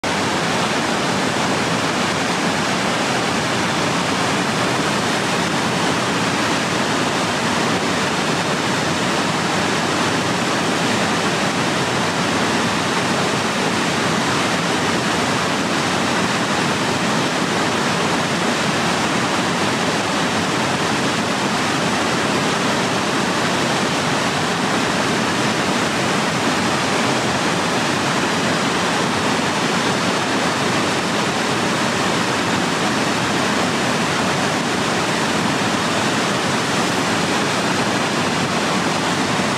House Rock Falls, video 3, 6k
This is House Rock Falls on the South Santiam River in Oregon.